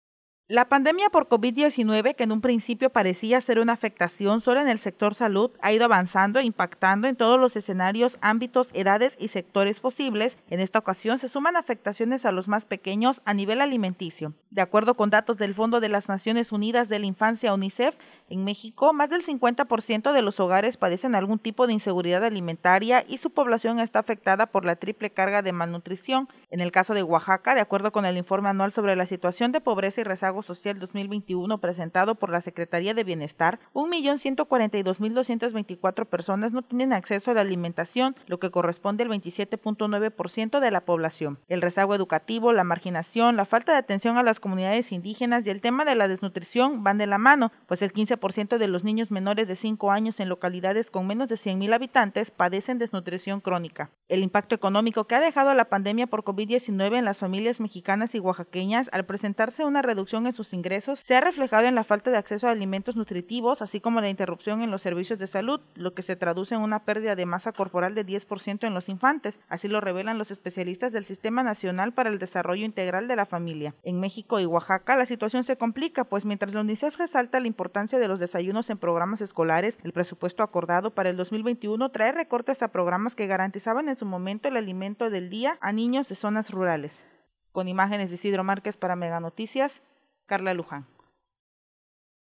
NP DESNUTRICION INFANTIL
Organizaciones, senadores y representantes de la población en las instituciones gubernamentales han advertido sobre el incremento de la desnutrición infantil como efecto secundario de la pandemia. Pese al incremento en las estadísticas, pocas son las acciones efectivas que se han tomado.